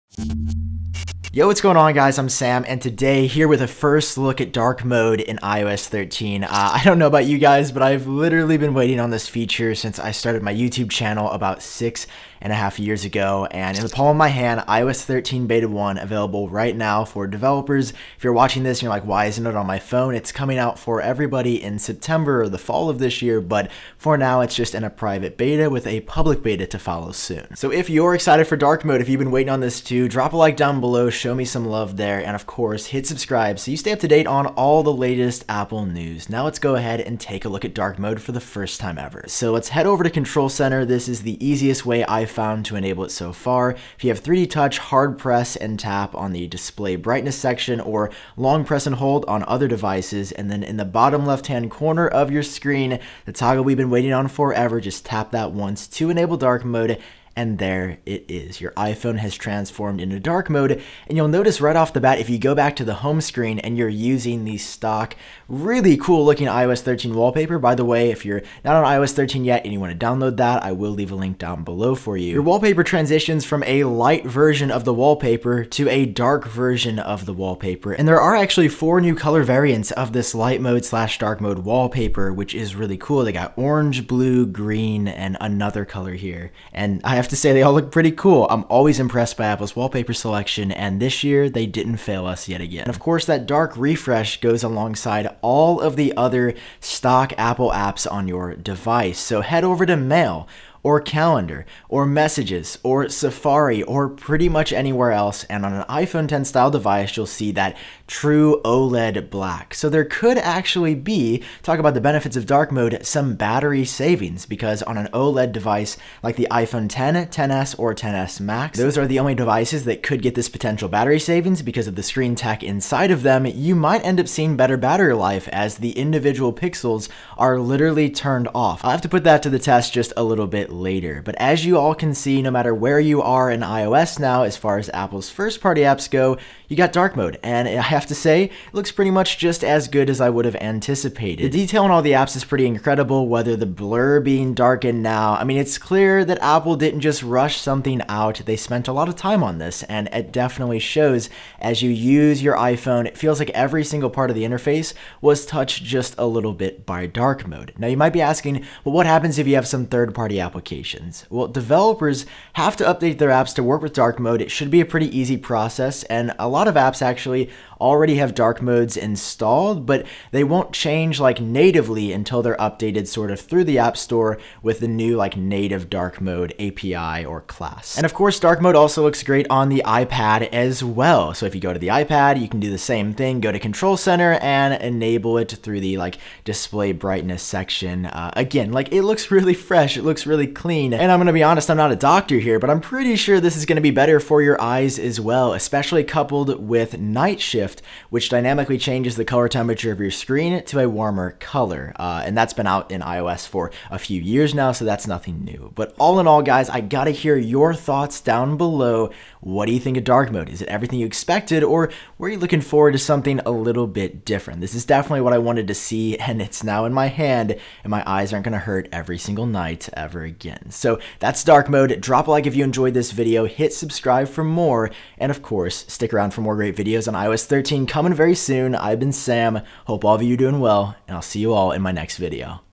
iOS 13 Hands-On: How To Enable Dark Mode!
After years of anticipation, iOS 13 finally introduces Dark Mode. And it's incredible. It makes the entire interface dark (what? no way?!?) and genuinely turns using your iPhone or iPad at night back into an enjoyable task. What do you think of Dark Mode?